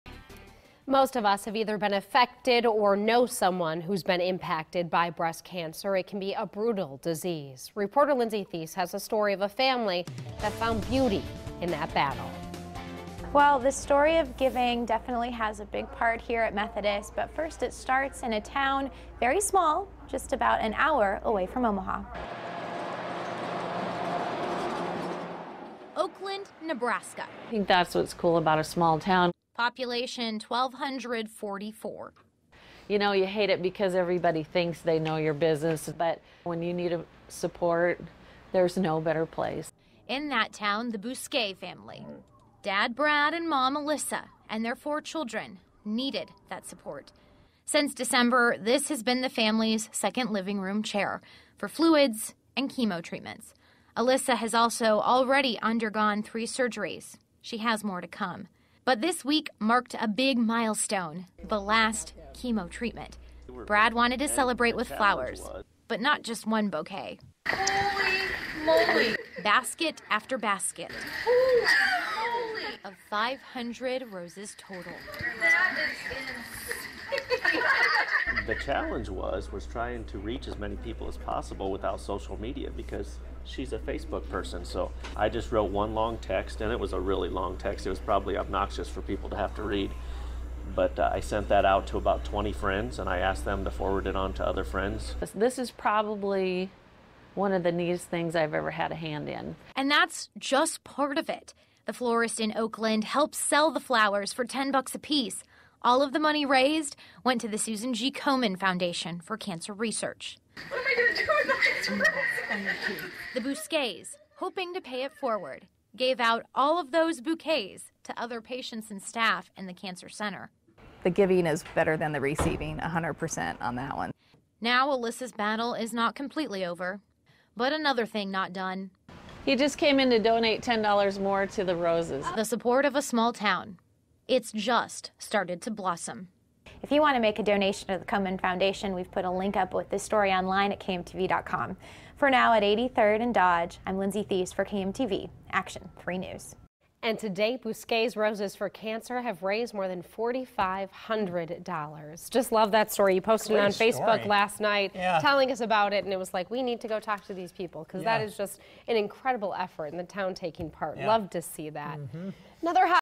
Husband gets wife 500 roses for last chemo
Most have us have either been affected or know some one whose been impacted by breast cancer. It is a brutal disease.
One  family that found beauty in that battle.